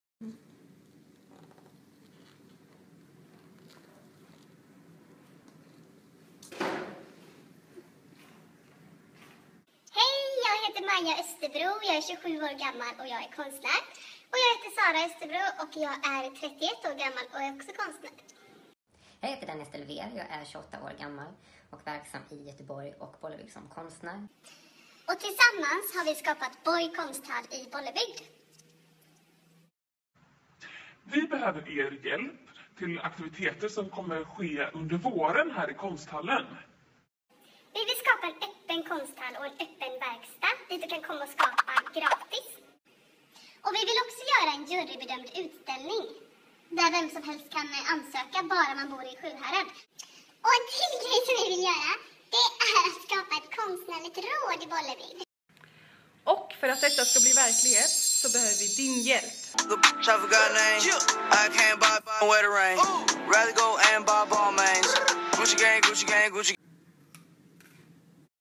Boy konsthall behöver din hjälp!
Vi vill skapa en öppen konsthall där delaktighet och undersökandet står i fokus, en levande mötesplats och en öppen oas. Vi behöver nu hjälp med att få drömmen att bli sann! Vi planerar ett antal aktiviteter under våren 2018, som ska sätta grunden för hur konsthallen ska utvecklas framöver. Nu behöver vi stöd från andra som också vill se en öppen och deltagarbaserad konsthall i Bollebygd!

Vad vi ska göra:

Vi samlar ett konstnärligt råd
Vi vill samla en grupp engagerade människor boende i Bollebygd. Gruppen ska ha funktionen av ett konstnärligt råd som tillsammans rådlägger kring programläggning, strategier för framtiden och som sätter konsthallens riktning.

Vi arrangerar 10 öppna verkstäder-tillfällen
Tillsammans med en konstnärs stöttning skapar du din egna konst. Vi har materialet - du har idéerna. Arbeta fritt och känn dig som hemma. Här kan du utmana din kreativitet och arbeta med material som du kanske annars inte hade prövat. Det finns inga rätt eller fel - bara lust och passion. Här kan alla komma för att testa akvarell för första gången, prova på mosaik, action-painting, grafitti eller göra sina egna smycken. Gör verklighet av dina galnaste idéer! Här får du vara konstig, explosiv, tyst, för dig själv, med andra - som du är.

Vi skapar 2 konst- och designmarknader
Två marknader som visar konst- och designhantverk från spännande konstnärer. Här hittar du små kollektioner med unika designer.

Vi drar ihop en stor vårutställning
En jurybedömd utställning öppen för både yrkesverksamma konstnärer som amatörer från Sjuhärad. Efter öppen utlysning väljs 10 konstnärer ut att delta. Salongen ska visa på en bredd av uttryck och lustfyllt skapande, kvalitet och personlighet, och ska visa vilken kraft som finns i Sjuhärad. Alla kan skicka in sitt bidrag!

STÖD BOY KONSTHALL
SWISHA OSS: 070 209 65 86
MÄRK MED "BOY KONSTHALL"

TACK!